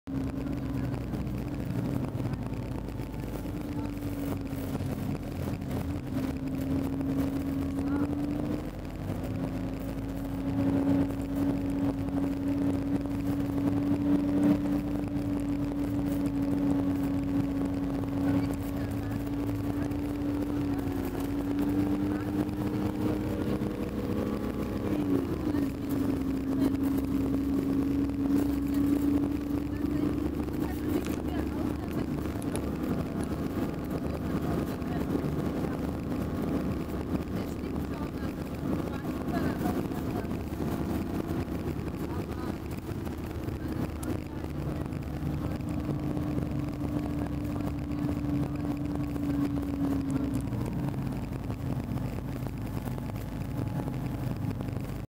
UNIS bandwagon in Adventdalen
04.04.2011 con un mezzo cingolato (bandwagon) ci dirigiamo dall'Adventdalen verso Todalen